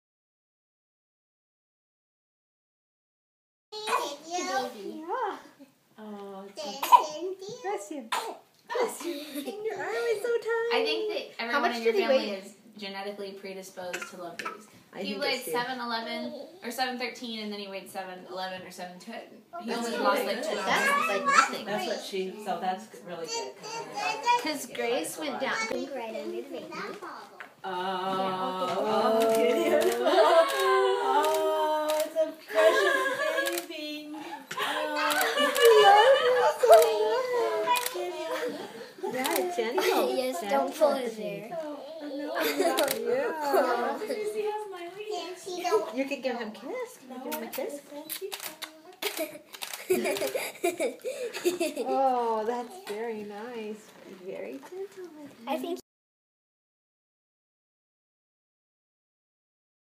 Gideon with Noah
Gideon holding Noah for the first time